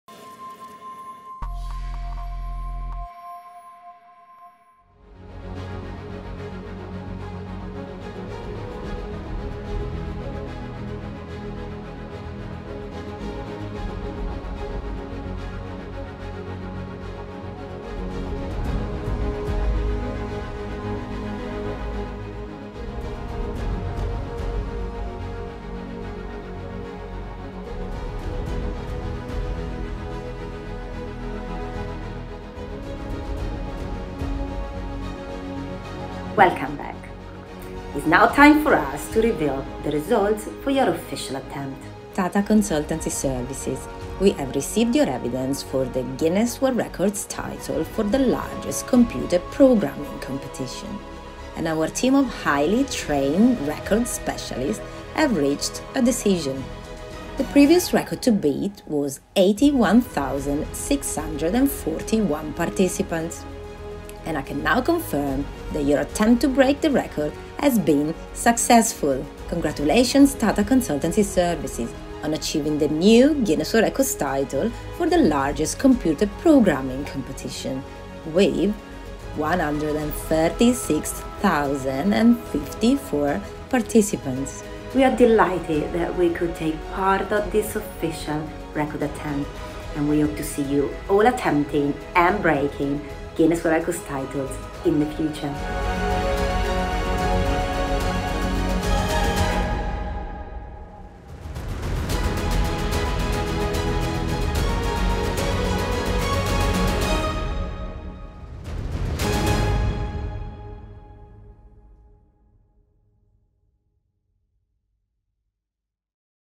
TCS wins Guinness World Records™
"The largest computer programming competition consists of 136,054 participants, and was organised by Tata Consultancy Services (India), in Mumbai, Maharashtra, India, on 10 April 2021."
This is TCS’ maiden feat at the Guinness World Records™. CodeVita, TCS' Flagship Coding Contest, is one of the pioneering technology events that has now become a global phenomenon. It has spearheaded “Programming as a Sport” among generations of creative and passionate millennial across the world. A joint venture of TCS Talent Acquisition and TCS Research & Innovation unit, CodeVita has united coders from diverse backgrounds, cultures and ethnicities, revolutionized the talent ecosystem, and propelled innovation and change.

Registrations for Season 10 are about to open soon.